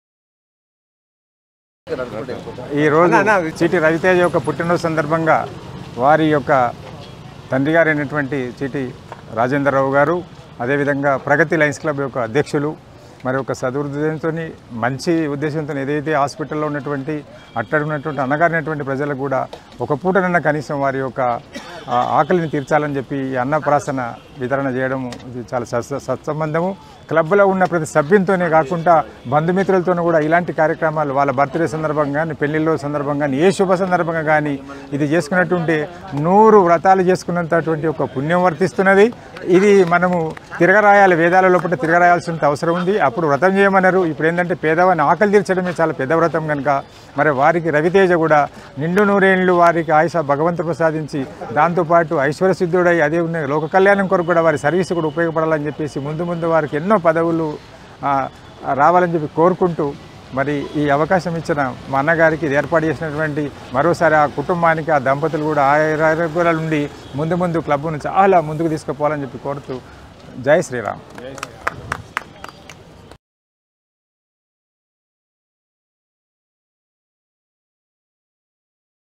Cheeti Raviteja | Birthday Celebrations | Cheeti Rajender Rao | Lions Club | Karimnagar
శ్రీ చీటి రవితేజ గారి జన్మదినం సందర్బంగా
Sri Cheeti Raviteja's Birthday Celebrations
S/o. Cheeti Rajender Rao garu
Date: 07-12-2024

లయన్స్ ఇంటర్నేషనల్ జిల్లా 320G ఆధ్వర్యంలో  మీల్స్ ఆన్ వీల్స్ ద్వారా
ఉచిత అన్న ప్రసాద వితరణ కార్యక్రమం
స్థలం: ప్రభుత్వ ఆసుపత్రి ప్రాంగణంలో 200 మందికి పైగా అవసరార్థులకు అన్నప్ర సాద వితరణ చేయడము జరిగినది.

మరియు కార్యక్రమం లో పాల్గొన్న:
లయన్ శ్రీ చీటి రాజేందర్ రావు గారు
లయన్ శ్రీ జువ్వాడి రంగా రావు గారు
లయన్ శ్రీ రావికంటి కృష్ణ కిశోర్ గారు
లయన్ శ్రీ చీటి ప్రతాప్ రావు గారు
శ్రీ  ఎనగంటి సురేందర్ రావు గారు
శ్రీ తోట శ్రీనివాస్ రావు గారు
శ్రీ పి. శ్రీహరి రెడ్డి గారు
లయన్ శ్రీ జోగినపల్లి రఘునందన్ రావు గారు